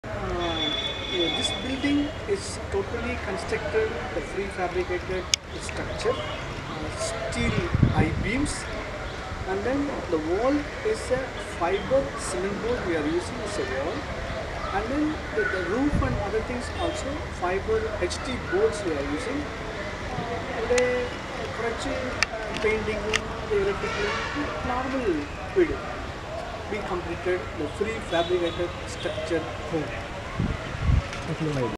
WHAT ARE THE MATERAILS USED TO BUILD PRE FAB HOMES?- FAQ ANSWERS
പ്രീ- ഫാബ് വീടുകൾ നിർമ്മിക്കുന്നതിന് MATERAILS ഉപയോഗിക്കുന്നത് എന്താണ്?WHAT ARE THE MATERAILS USED TO BUILD PRE FAB HOMES?-FAQ - ANSWERS BY EXPERTS. Frequently Ask Question about Pre Fab Home ... we are here given the answers.